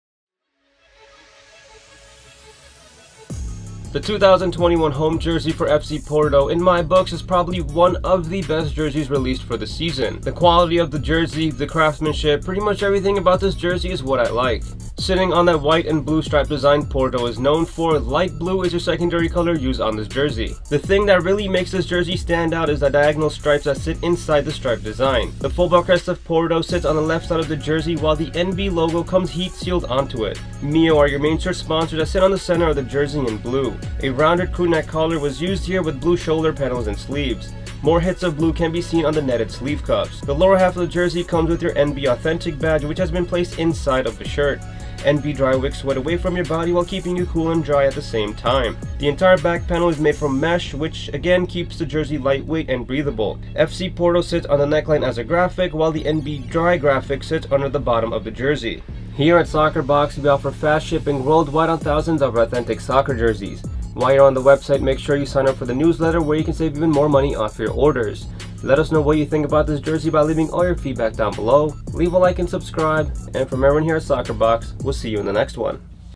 Porto Home Football Shirt 21/22
The Porto home jersey is new this season and is available today at Soccer Box!

Official New Balance soccer jersey at a great price.